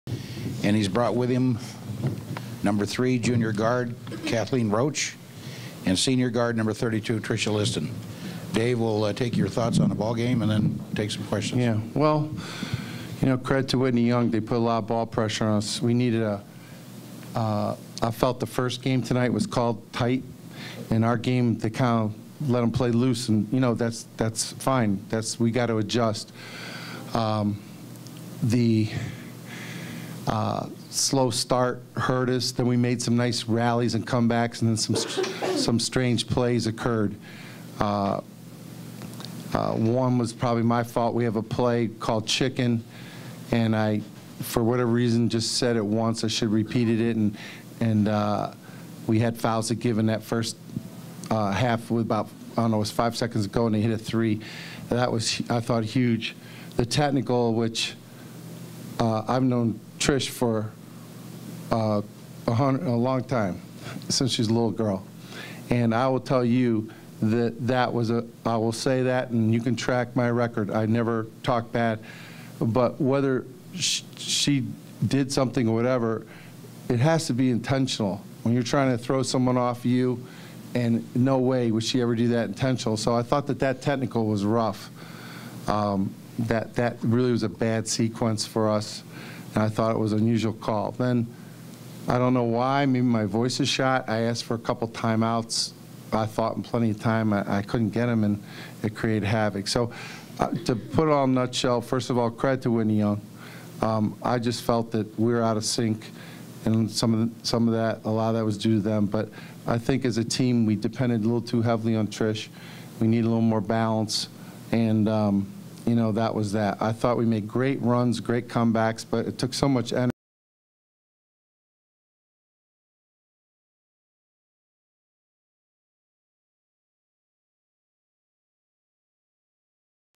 Illinois Prep Fenwick Girls Basketball Post Game
Coach Dave Power talks at press conference about the loss to Whitney Young in the State Semifinals.